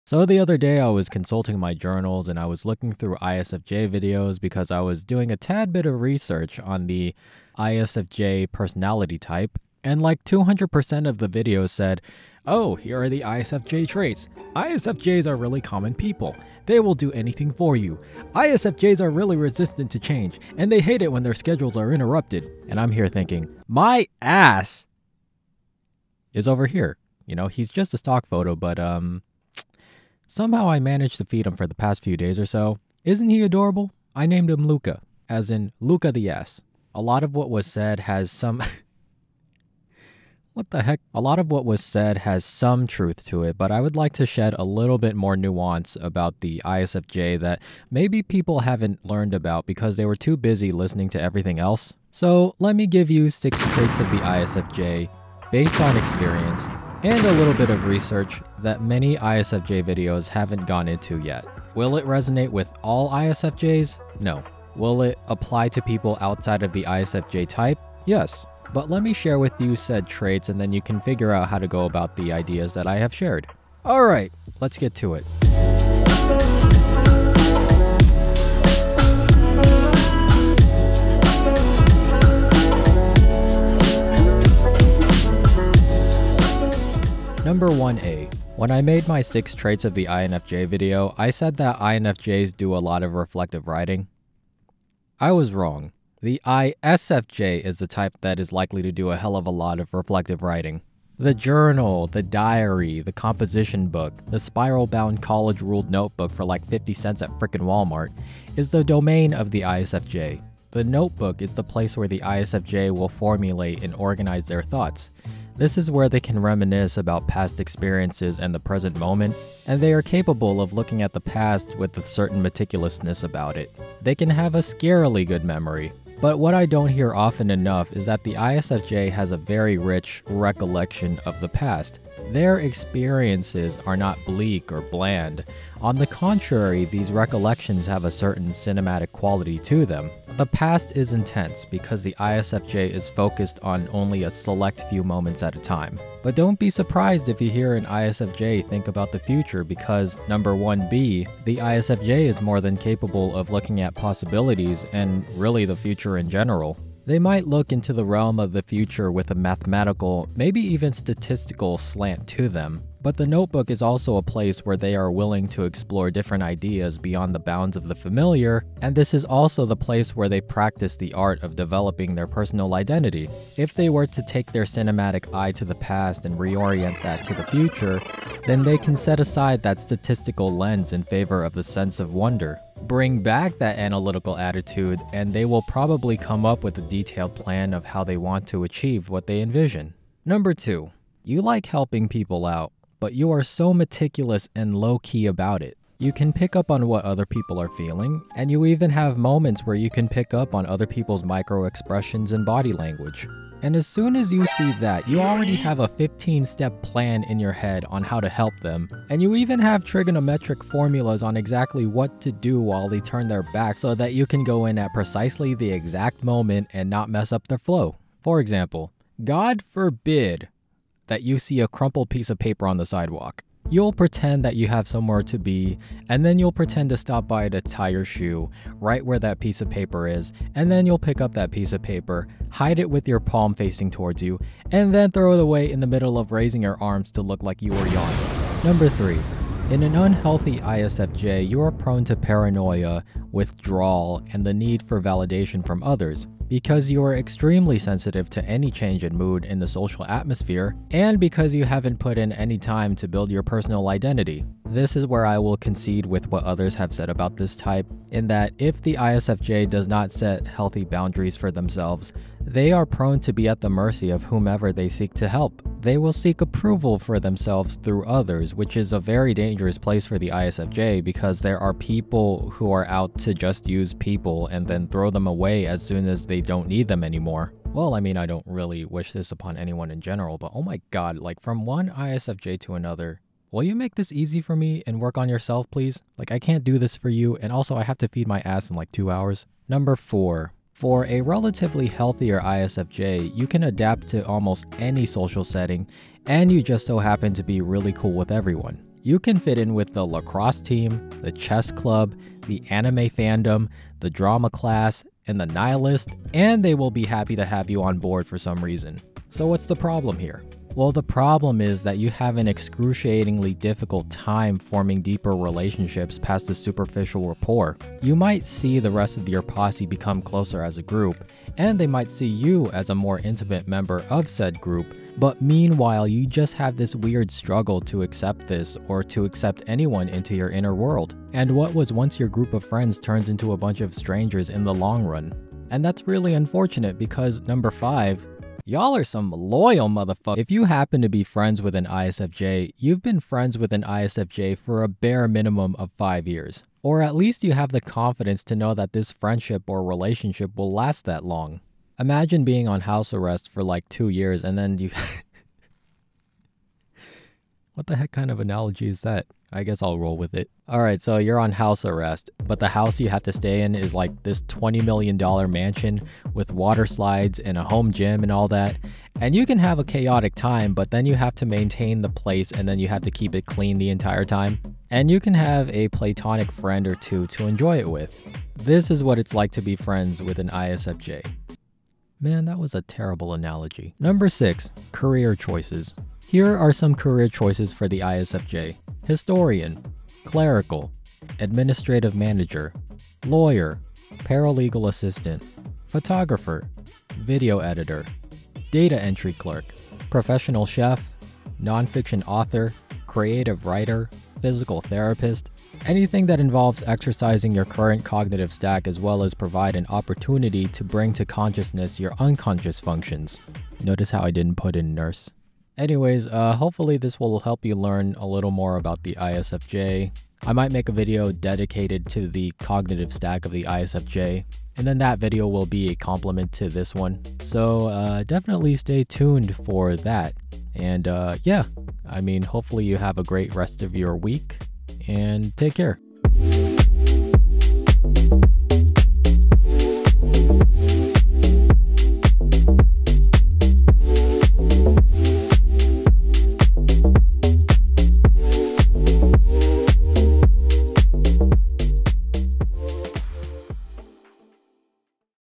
6 Traits of the ISFJ That Even ISFJs Should Look At
Are you an ISFJ? Are you an ISFJ that might be mistyped as an INFJ, ISTJ, ISFP, ESFP, ESPN, ESFJ, or some other type?

Then this video may or may not help you. Eh, who knows, you might learn something out of this? But these are the traits of the ISFJ, coming from an ISFJ main, INFJ alt.

Or if you want to get a good laugh or two, this video might do it.

I’m just a layman why the heck are you going to me for knowledge? Lol

Here are some timestamps if you want to snoop around:

0:00 Intro
1:16 Intro and Writing
2:32 Future Thinking
3:10 Helping
3:58 Paranoia
4:50 Superficial Relaitonships
5:37 Loyalty
6:21 Career Choices

Here are some sources I’ve looked into, that stood out to me, to help me articulate more about the ISFJ type:

Here are some tunes to look into (support the artists!)